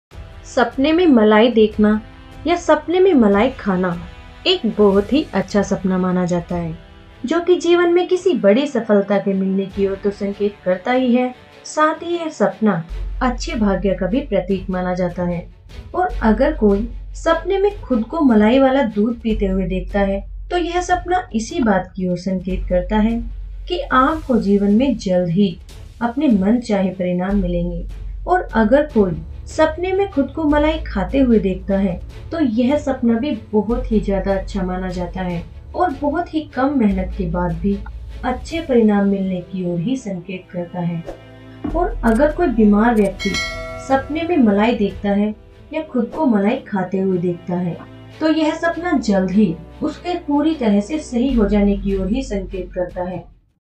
Sapne me malai khana | eating cream in dream | sapne me malai dekhna kaisa hota hai
Sapne mein malai dekhna ya khana kitna shubh ya ashubh hota hai.